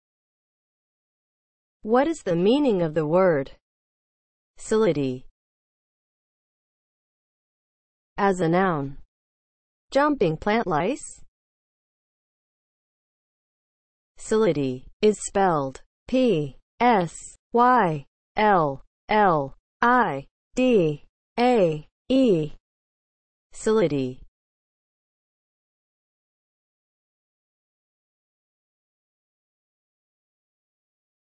What is the meaning of the word PSYLLIDAE?
Definition and spelling of the word PSYLLIDAE.

(00:00) Intro
(00:07) As a noun
(00:13) Spelling

As a noun (00:07):
• jumping plant lice